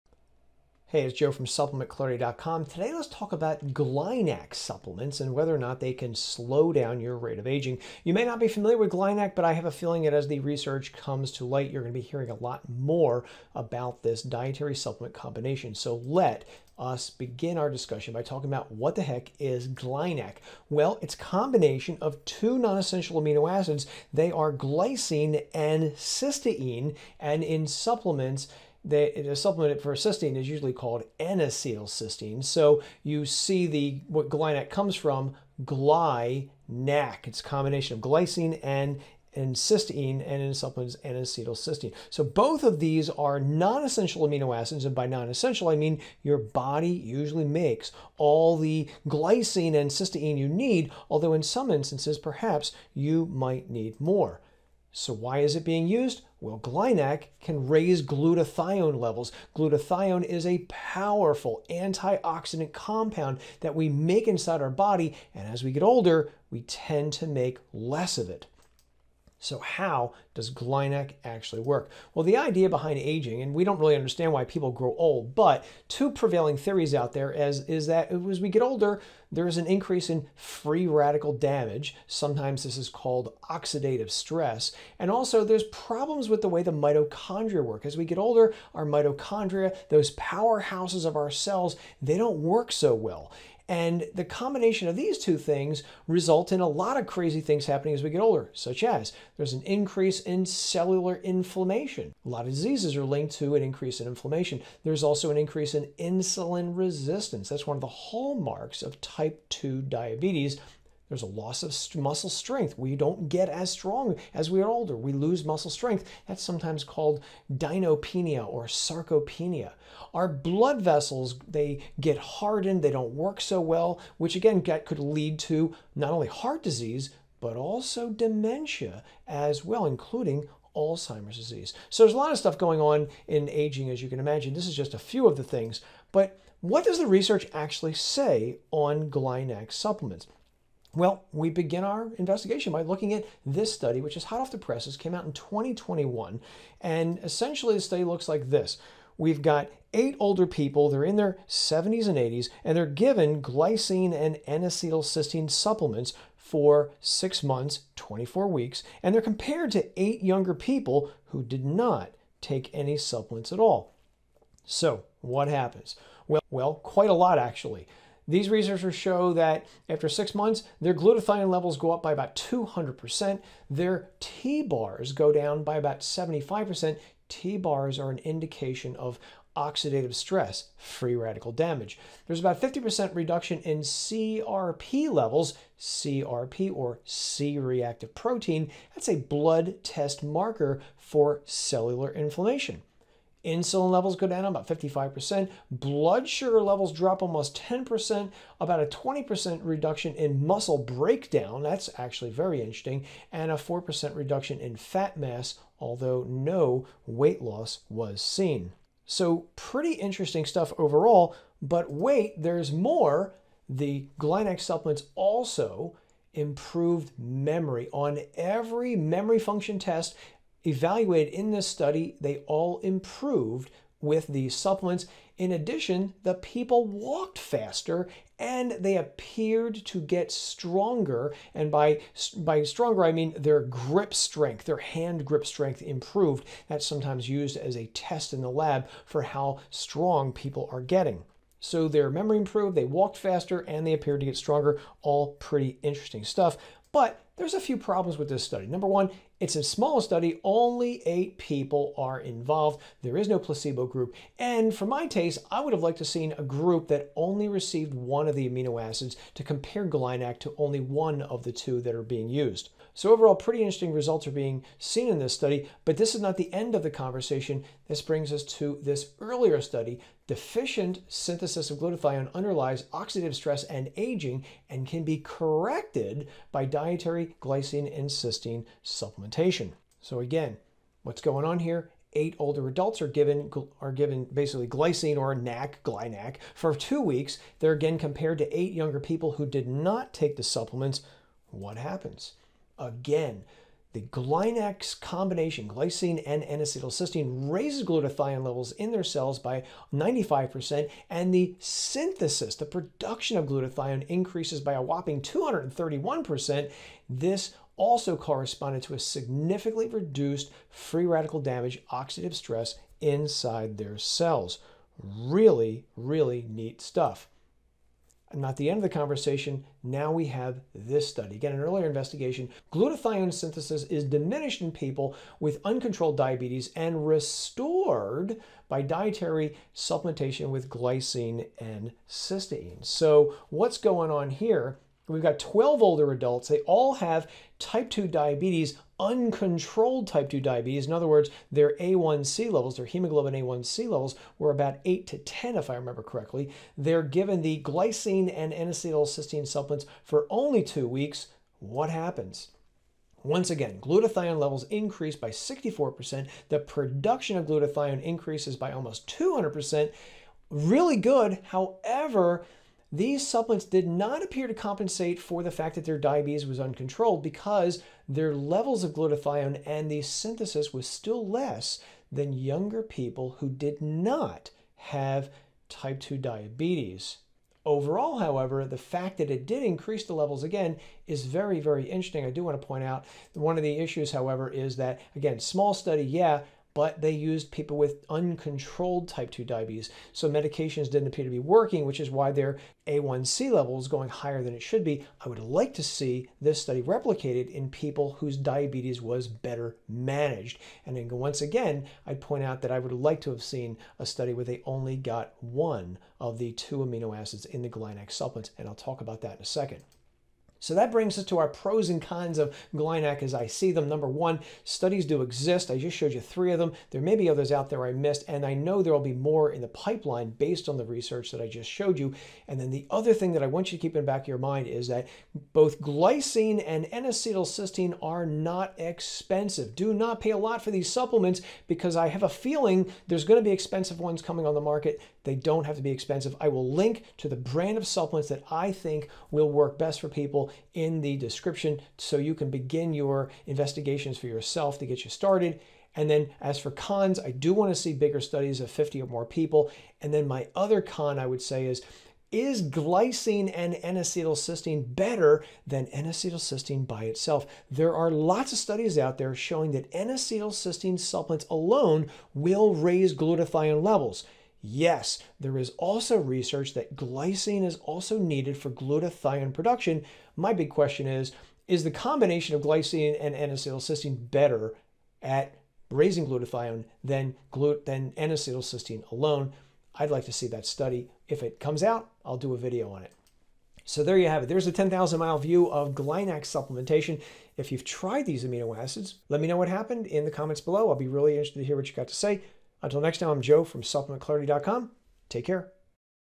GlyNac Supplements: The Missing Anti-Aging Nutrients?
GlyNAC is the combination of the amino acid glycine and N-acetylcysteine (NAC), both of which help with the production of glutathione, sometimes called the master antioxidant enzyme in our cells. Glutathione tends to decline as we get older. Several human and animal studies have linked GlyNac supplements to higher glutathione levels, lower inflammation, and improved muscle strength. This video summarizes the results of another GlyNac clinical trial involving 24 older adults who took the supplement for up to 16 weeks.

00:00 Introduction
00:18 What is GlyNAC?
01:10 How Does GlyNAC Work?
02:19 GlyNAC Research Study 1
04:29 GlyNAC Research Study 2
05:19 GlyNAC Research Study 3
06:51 GlyNAC Pros & Cons

🔴🔴 🔴Things used to make this video:
1 Lot’s of work
2 This video editing software
3 This Microphone